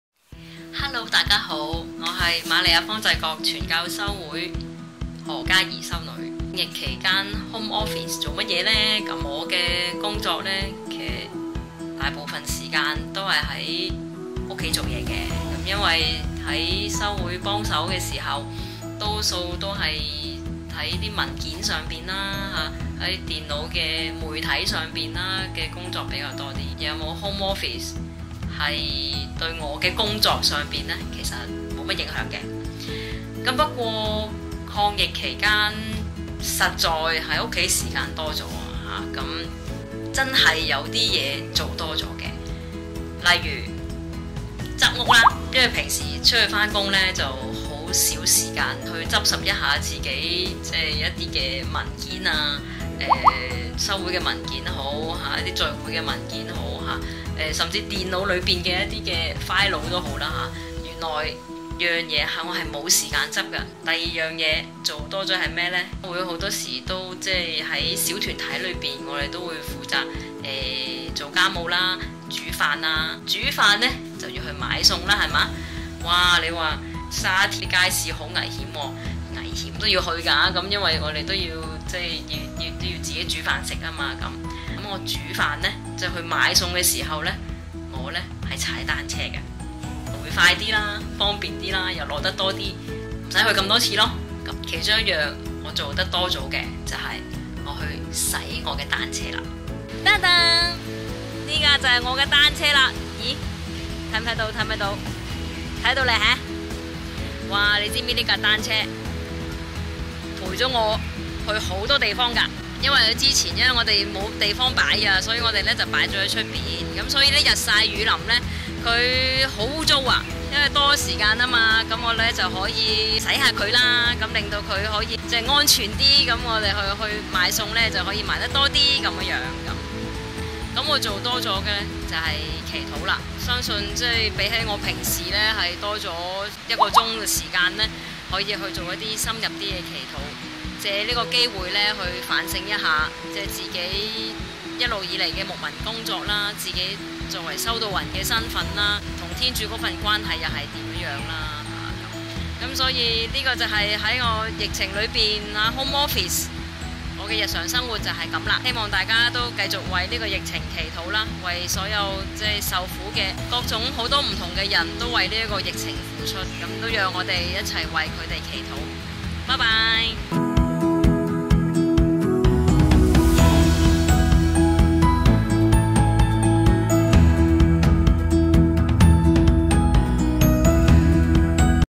San修女【神父修女 HOME OFFICE❓】沸點Special
瑪利亞方濟各傳教修會何家怡 修女，在家工作 期間可能都同我哋一樣：執屋！執！執！執！另外，修女都要煮飯、仲踩單車去買餸嘅！修女 HomeOffice 多左時間祈禱⋯⋯等我地都多啲祈禱 抗疫 啦！

😷疫情嚴峻，沸點 陪你 WorkFromHome 📲🗂！為神父、修女、獻身生活者💒Home即是Work，Work即是Home💒⁉️ 佢地最近嘅生活又係點呢❓